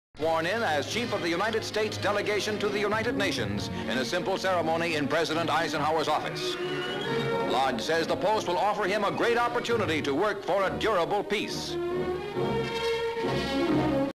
Lodge Takes Oath (1953)
Item title reads - 'Lodge takes oath as top U.N. delegate.'

Washington D.C., America (USA).

M/S as Senator Henry Cabot Lodge takes oath as Chief of U.S. delegation to United Nations in front of President Dwight (Ike) Eisenhower. C/U cameraman filming them. M/S Lodge shaking hands with Eisenhower.
 FILM ID:2042.14